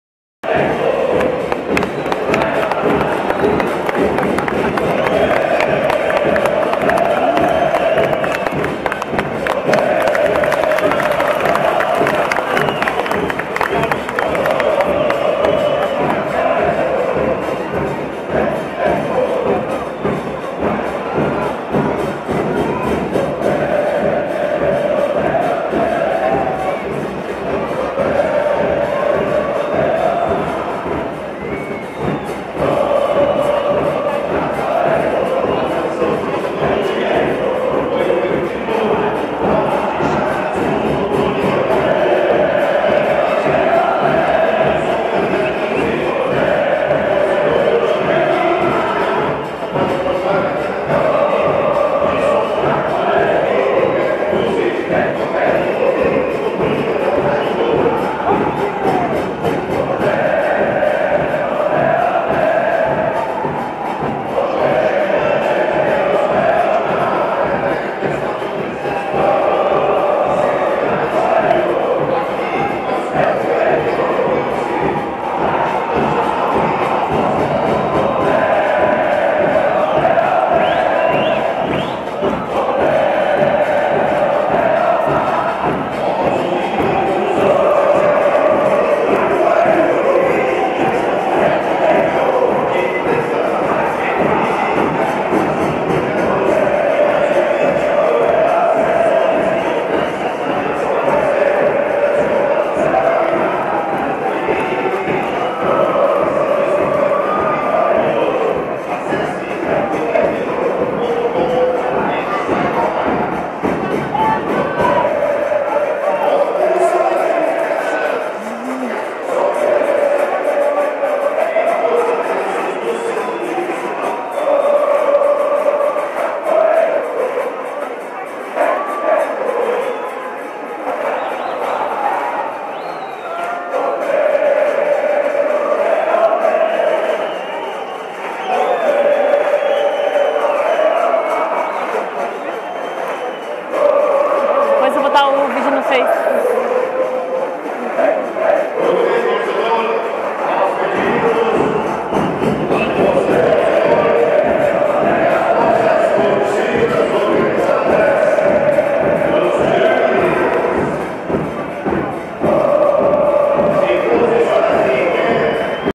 Desimpedidos em campo com o deus Paulo Baier ;)
Desimpedidos entrando em campo com o todo poderoso, meu ídolo Paulo Baier...Pela rodada #14 do brasileiro CriciúmaxCruzeiro 09/08/2014 Criciúma City